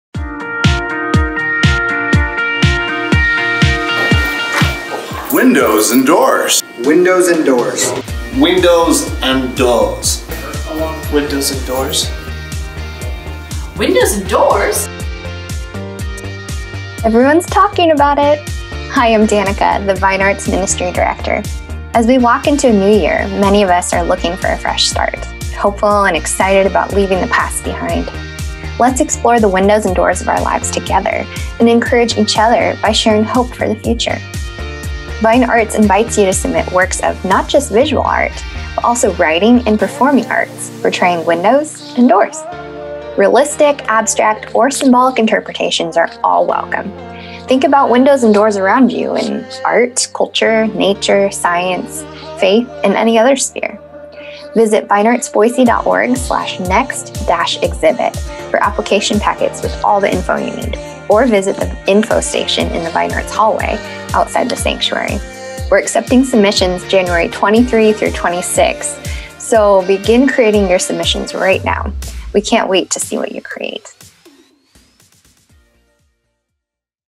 New VineArts Exhibit
VineArts invites creatives of all ages and skill levels to submit works of visual, writing, and performing art portraying windows and doors.

Realistic, abstract, or symbolic interpretations are all welcome. Think about windows and doors around you in art, culture, nature, science, spirituality, or any other sphere. for application packets with all the info you need. Or visit the info station in the VineArts hallway outside the sanctuary.

We’re accepting submissions January 23-26, 2022, so begin creating your submissions now!